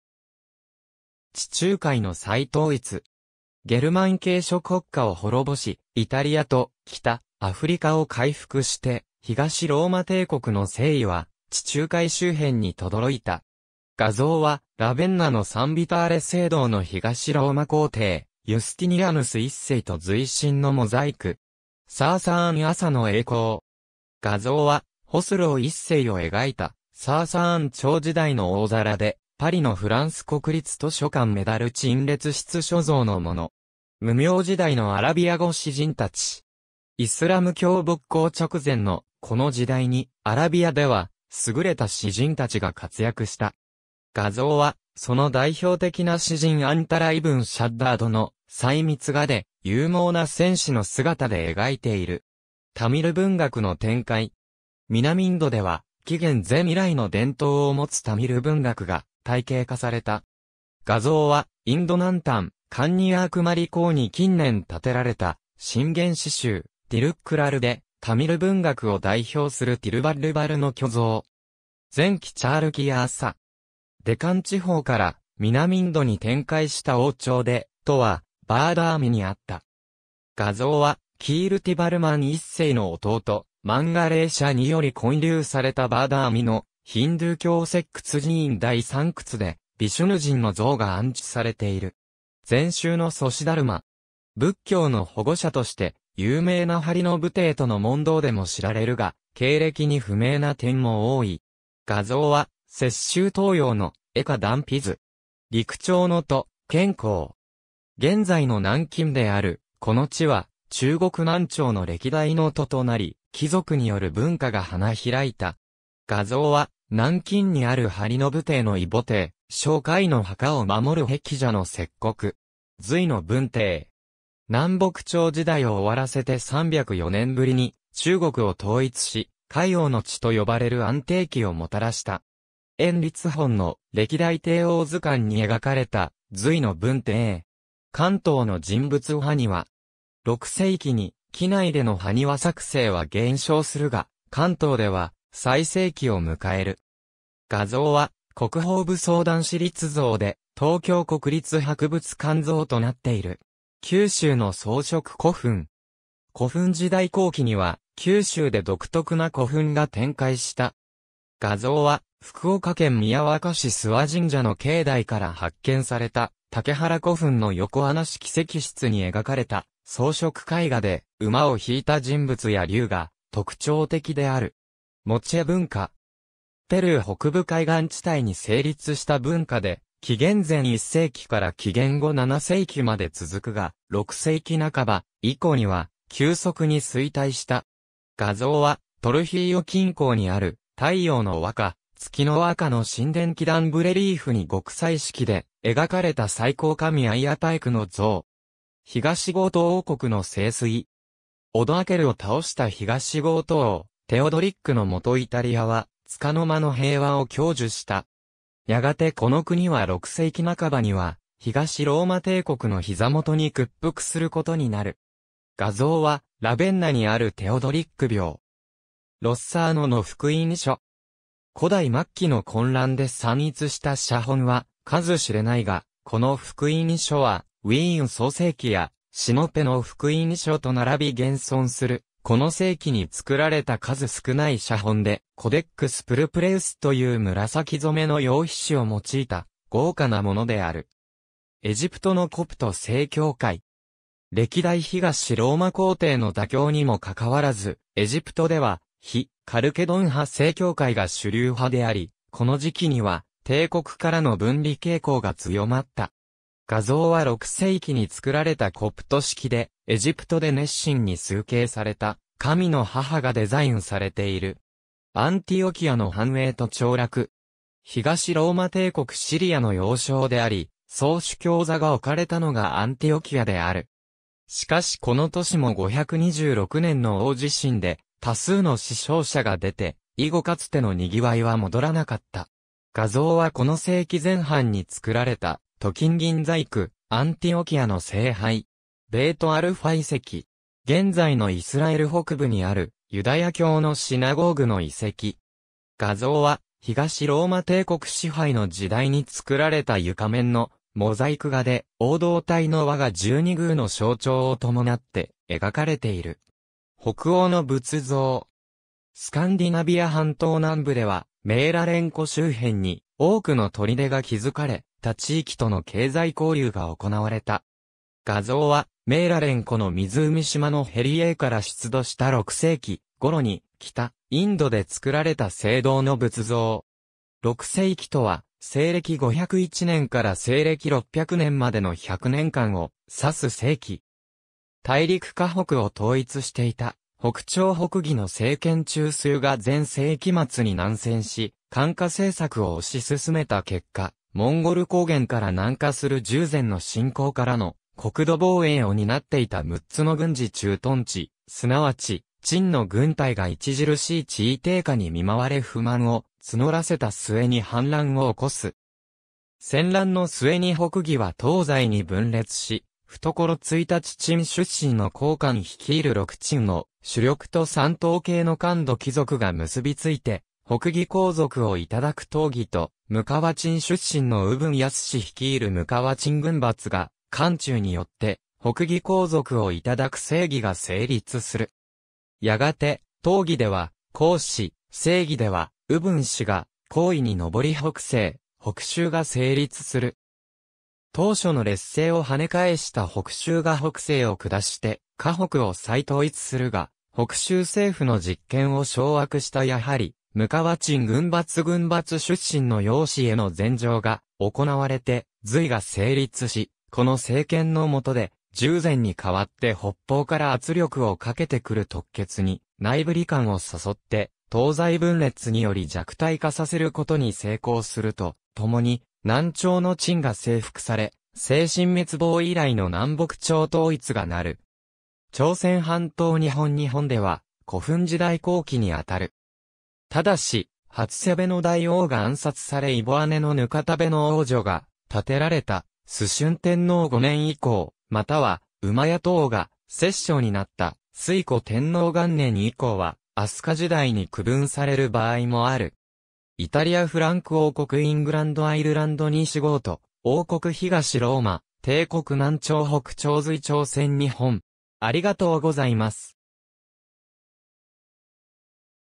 6世紀
地中海の再統一。
ゲルマン系諸国家を滅ぼし、イタリアと北アフリカを回復して、東ローマ帝国の勢威は地中海周辺に轟いた。
画像はラヴェンナのサン・ヴィターレ聖堂の東ローマ皇帝ユスティニアヌス1世と随臣のモザイク。
サーサーン朝の栄光。
画像はホスロー1世を描いたサーサーン朝時代の大皿で、パリのフランス国立図書館メダル陳列室（コイン・メダル博物館）所蔵のもの。
無明時代のアラビア語詩人たち。
イスラム教勃興直前のこの時代にアラビアでは優れた詩人たちが活躍した。
画像はその代表的な詩人アンタラ・イブン・シャッダードの細密画で、勇猛な戦士の姿で描いている。
タミル文学の展開。
南インドでは紀元前以来の伝統を持つタミル文学が体系化された。
画像は インド南端カンニヤークマリ港に近年建てられた、箴言詩集『ティルックラル』でタミル文学を代表するティルヴァッルヴァルの巨像。
前期チャールキヤ朝。
デカン地方から南インドに展開した王朝で都はバーダーミにあった。
画像はキールティヴァルマン１世の弟マンガレーシャにより建立されたバーダーミのヒンドゥー教石窟寺院第3窟でヴィシュヌ神の像が安置されている。
禅宗の祖師達磨。
仏教の保護者として有名な梁の武帝との問答でも知られるが、経歴に不明な点も多い。
画像は雪舟等楊の「慧可断臂図」（愛知県常滑市斉年寺蔵）。
六朝の都建康。
現在の南京であるこの地は中国南朝の歴代の都となり、貴族による文化が花開いた。
画像は南京にある梁の武帝の異母弟蕭恢の墓を守る辟邪の石刻。
隋の文帝。
南北朝時代を終わらせて300余年ぶりに中国を統一し、「開皇の治」と呼ばれる安定期をもたらした。
閻立本の「歴代帝王図巻（ボストン美術館蔵）」に描かれた隋の文帝。
関東の人物埴輪。
6世紀に畿内での埴輪作成は減少するが、関東では最盛期を迎える。
画像は「国宝武装男子立像（群馬県太田市出土）」で東京国立博物館蔵となっている。
九州の装飾古墳。
古墳時代後期には九州で独特な古墳が展開した。
画像は福岡県宮若市諏訪神社の境内から発見された竹原古墳の横穴式石室に描かれた装飾絵画で、馬を引いた人物や竜が特徴的である。
モチェ文化。
ペルー北部海岸地帯に成立した文化で、紀元前1世紀から紀元後7世紀まで続くが、6世紀半ば以降には急速に衰退した。
画像はトルヒーヨ近郊にある「太陽のワカ、月のワカ」の神殿基壇部レリーフに極彩色で描かれた最高神アイアパエクの像。
東ゴート王国の盛衰。
オドアケルを倒した東ゴート王テオドリックのもとイタリアはつかの間の平和を享受した。
やがてこの国は6世紀半ばには東ローマ帝国の膝下に屈服することになる。
画像はラヴェンナにあるテオドリック廟。
『ロッサーノの福音書』。
古代末期の混乱で散逸した写本は数知れないが、この福音書は『ウィーン創世記』や『シノペの福音書』と並び現存するこの世紀に造られた数少ない写本で、「コデックス・プルプレウス」という紫染めの羊皮紙を用いた豪華なものである。
エジプトのコプト正教会。
歴代東ローマ皇帝の妥協にもかかわらず、エジプトでは非カルケドン派正教会が主流派であり、この時期には帝国からの分離傾向が強まった。
画像は6世紀に作られたコプト織でエジプトで熱心に崇敬された「神の母（テオトコス）」がデザインされている（クリーブランド美術館蔵）。
アンティオキアの繁栄と凋落。
東ローマ帝国シリアの要衝であり、総主教座が置かれたのがアンティオキアである。
しかしこの都市も526年の大地震で多数の死傷者が出て、以後かつての賑わいは戻らなかった。
画像はこの世紀前半に作られた鍍金銀細工「アンティオキアの聖杯」（メトロポリタン美術館蔵）。
ベイト・アルファ遺跡。
現在のイスラエル北部にあるユダヤ教のシナゴーグの遺跡。
画像は東ローマ帝国支配の時代に作られた床面のモザイク画で黄道帯（ゾディアック）の輪が十二宮の象徴を伴って描かれている。
北欧の仏像。
スカンディナヴィア半島南部ではメーラレン湖周辺に多くの砦が築かれ、他地域との経済交流が行われた。
画像はメーラレン湖の湖島のヘリエーから出土した6世紀ごろに北インドで作られた青銅の仏像（スウェーデン国立歴史博物館蔵）。
6世紀（ろくせいき）とは、西暦501年から西暦600年までの100年間を指す世紀。
大陸 華北を統一していた北朝北魏の政権中枢が前世紀末に南遷し漢化政策を推し進めた結果、モンゴル高原から南下する柔然の侵攻からの国土防衛を担っていた六つの軍事駐屯地、すなわち鎮の軍隊が著しい地位低下に見舞われ不満を募ら...